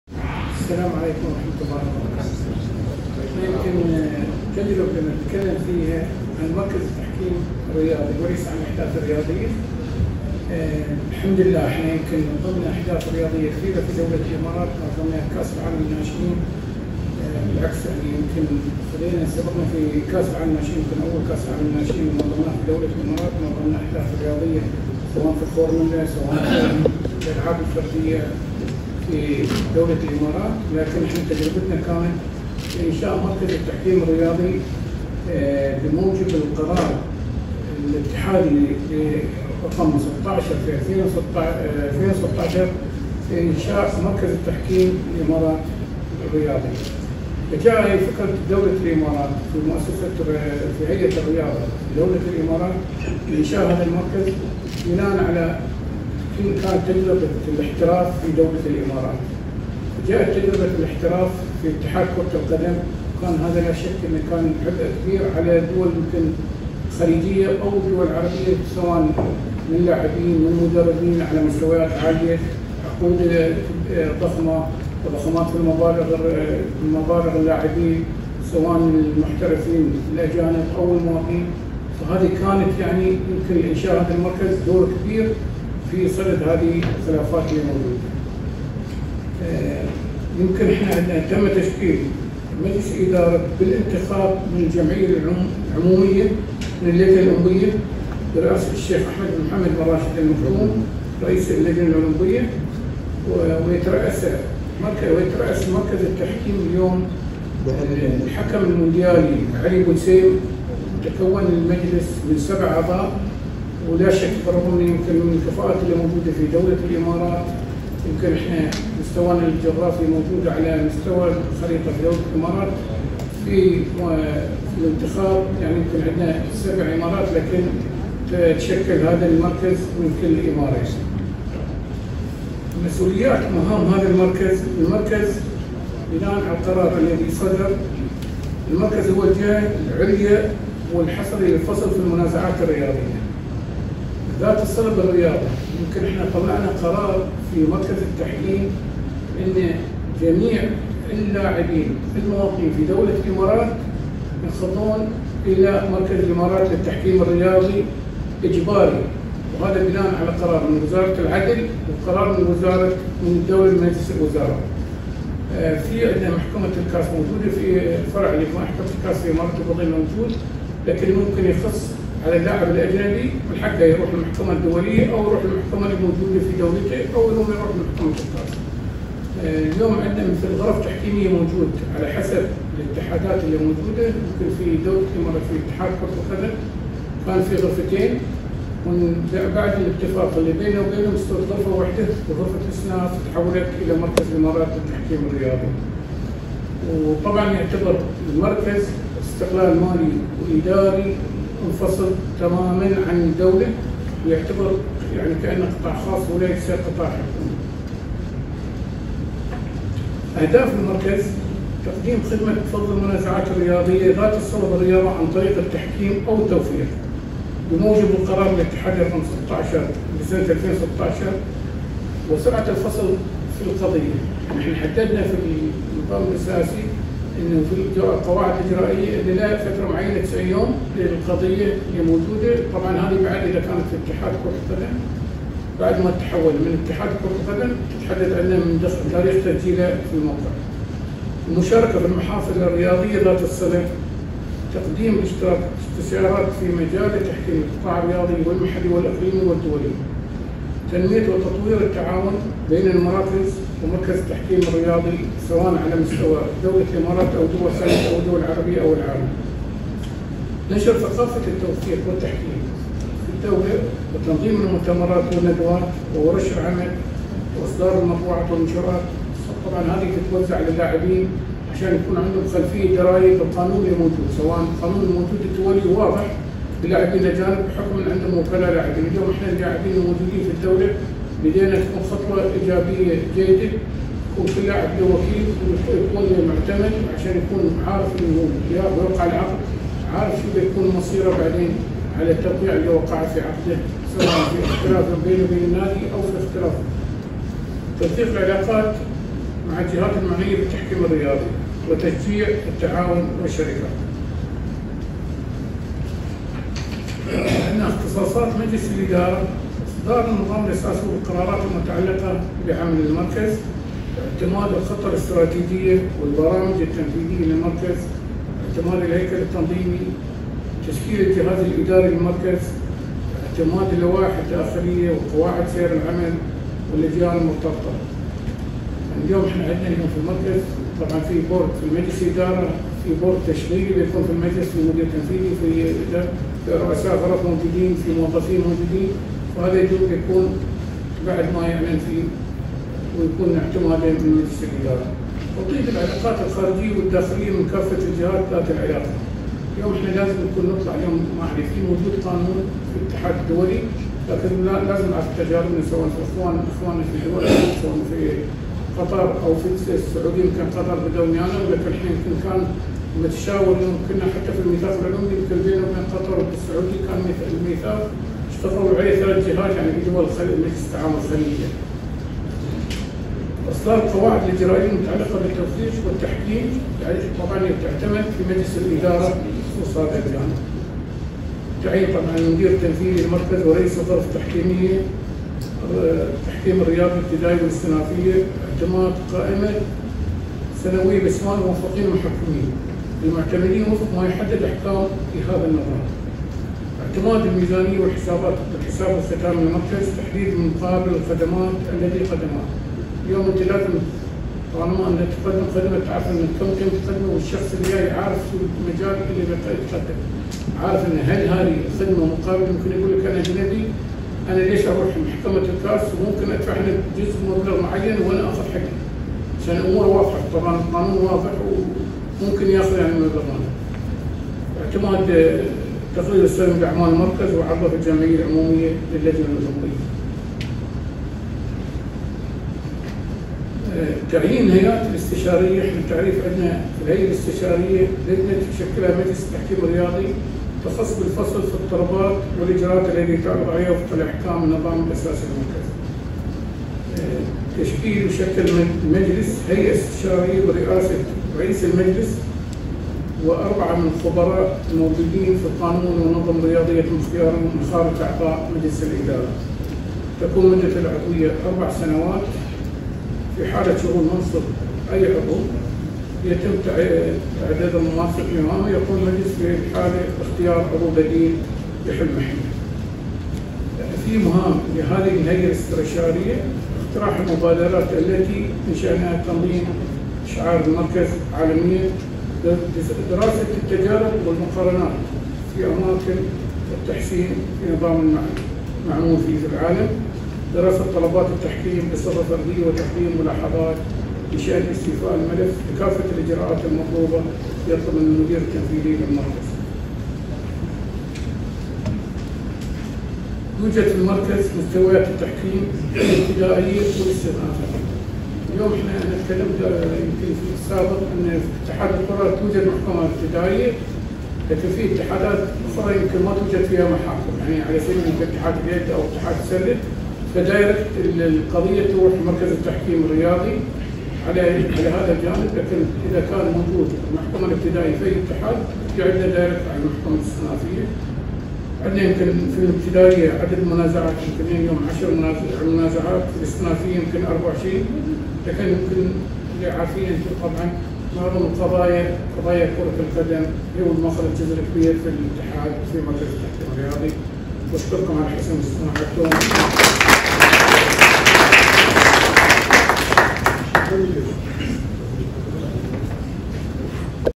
تجربة الإمارات في استضافة الأحداث الرياضية الكبرى يكشفها سعيد العاجل الشحي عضو مجلس إدارة مركز تحكيم
تجربة الإمارات في استضافة الأحداث الرياضية الكبرى يكشفها سعيد العاجل الشحي عضو مجلس إدارة مركز الإمارات للتحكيم الرياضي

تقدم لكم خلف_الحدث باقة متنوعة من أهم الأخبار التي تحدث في مصر، وتقدم لكم التفاصيل الكاملة لها.